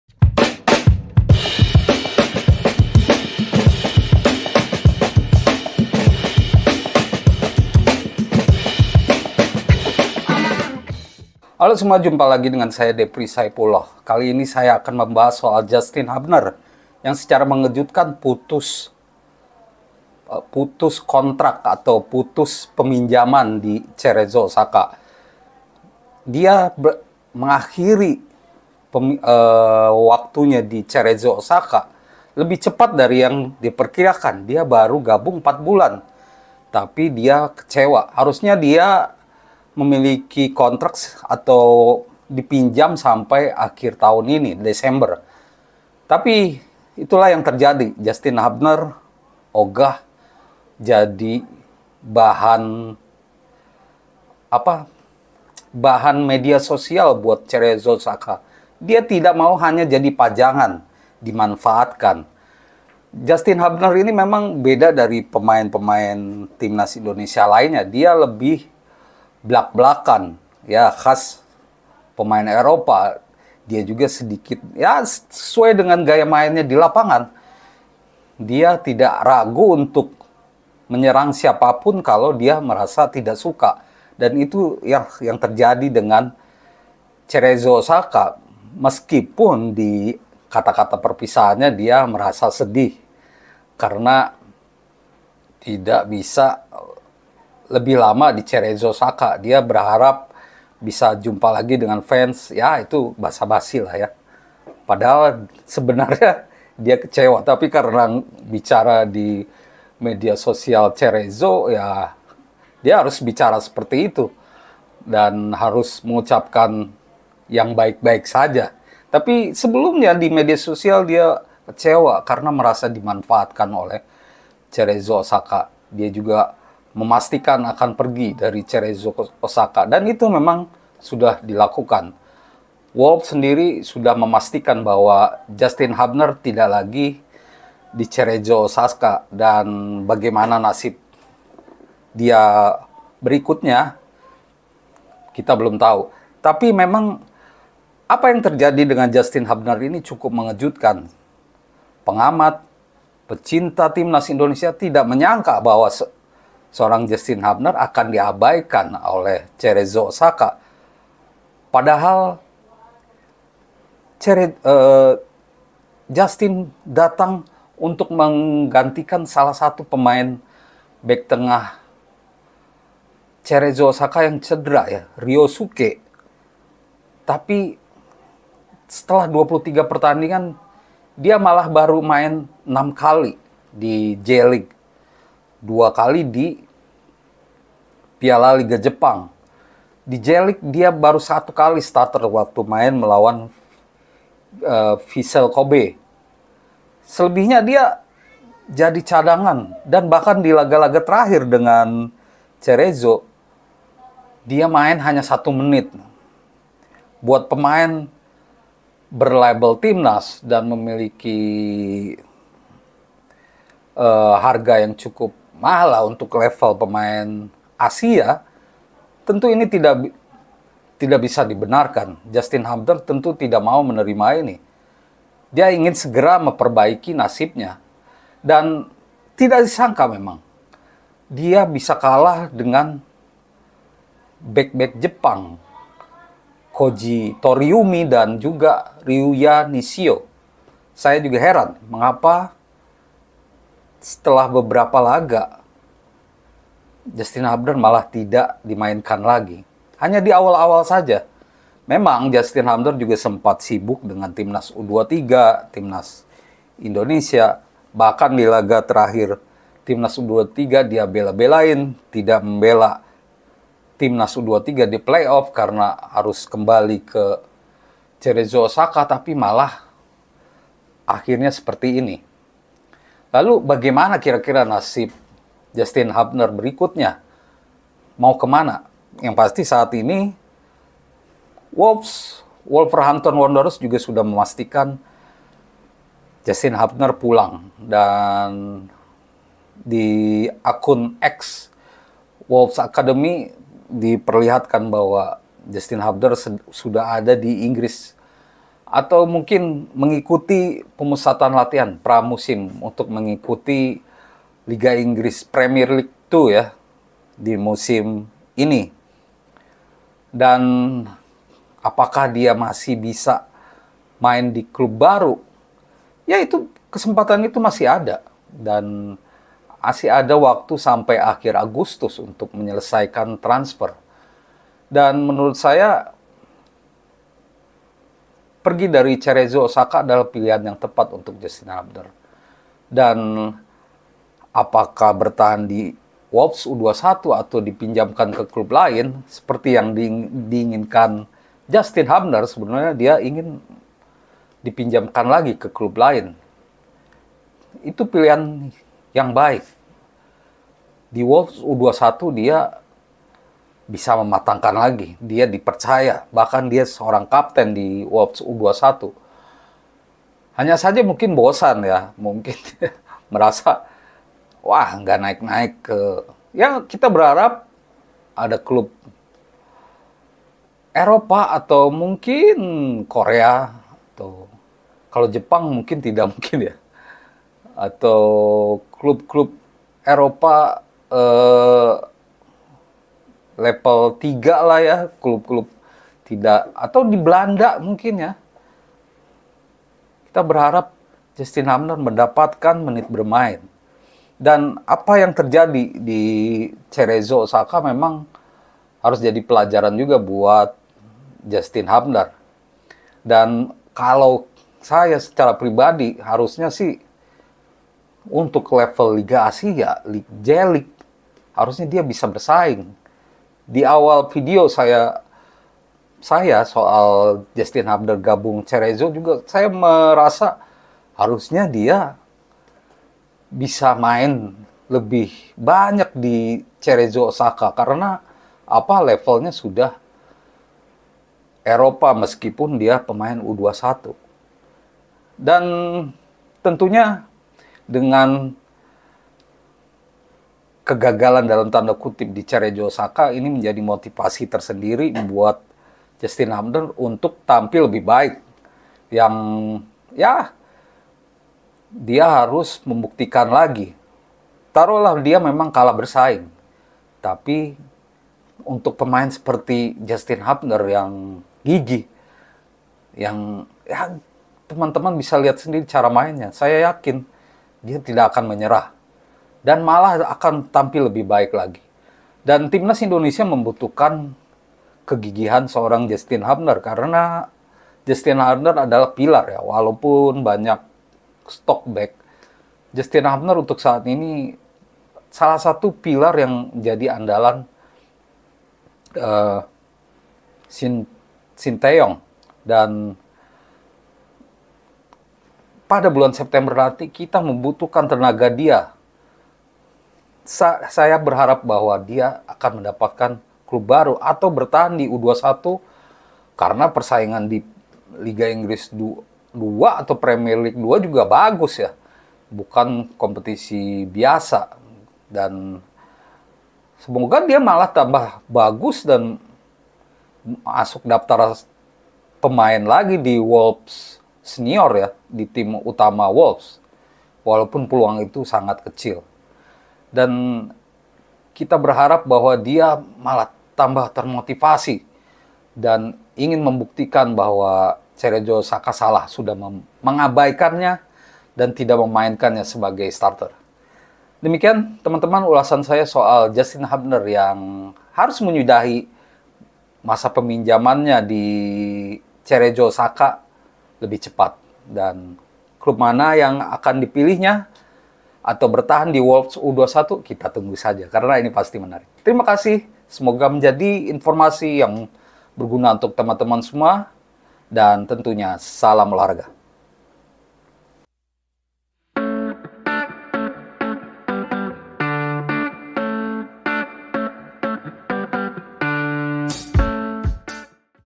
JUSTIN Hubner Sudah Benar Tinggalkan CEREZO Osaka, Tetap di Wolves U-21 atau Dapat Klub Lebih Baik?
Timnas Indonesia bakal terbantu kalau Justin Hubner dapatkan menit bermain yang banyak. Dia melawan, ogah main sedikit. Itulah yang dibutuhkan TIMNAS dan Hubner sekarang ini